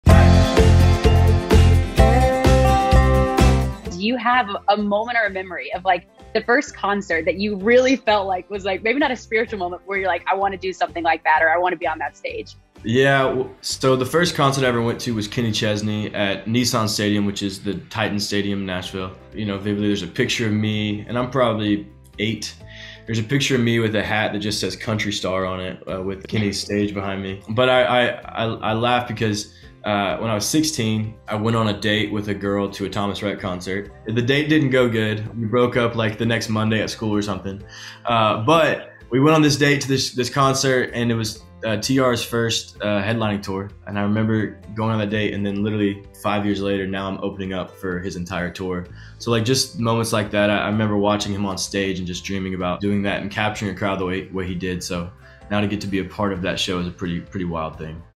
Conner Smith On How He First Met Thomas Rhett!
Conner Smith was able to video chat with our digital host Emily Curl. They talked about his music, meeting Thomas Rhett + more!

Listen to Conner's music here!

Follow Our Digital Host Emily Curl!

Listen Free to More of Your Favorite Country Artists on Your Phone!